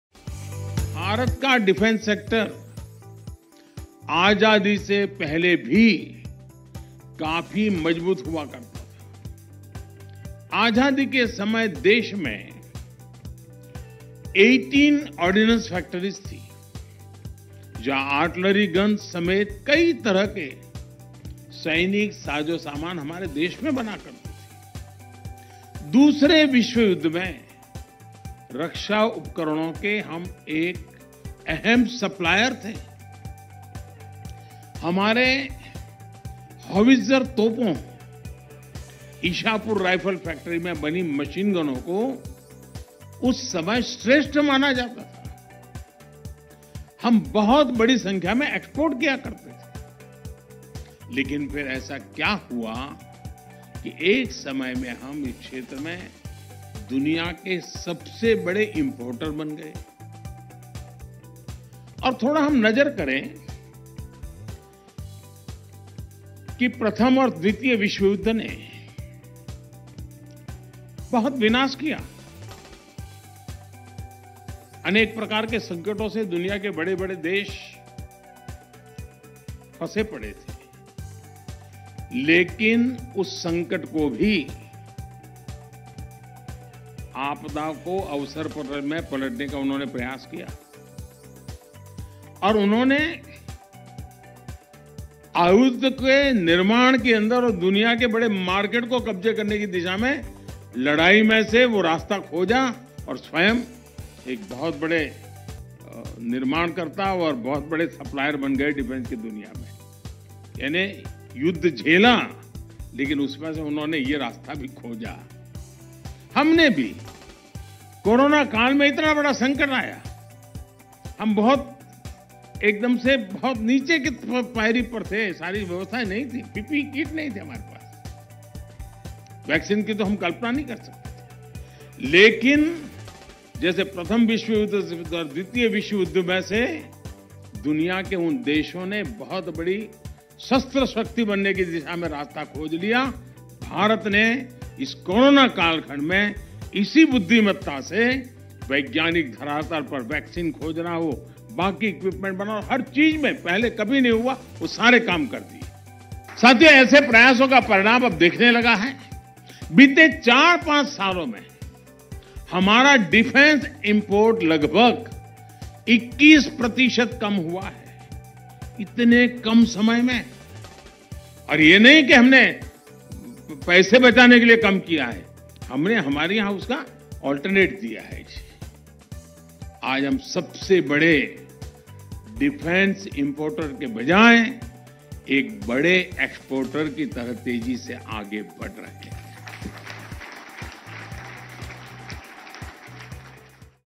आज हम सबसे बड़े डिफेंस इम्पोर्टर की बजाय एक बड़े एक्सपोर्टर की तरह तेजी से आगे बढ़ रहे हैं
हमारी होवित्जर तोपों, इशापुर राइफल फैक्ट्री में बनी मशीनगनों को श्रेष्ठ माना जाता था

हम बड़ी संख्या में Export किया करते थे लेकिन ऐसा क्या हुआ कि हम इस क्षेत्र में दुनिया के सबसे बड़े Importer बन गए?

बीते 4-5 वर्षों में हमारा डिफेंस Import लगभग 21% कम हुआ है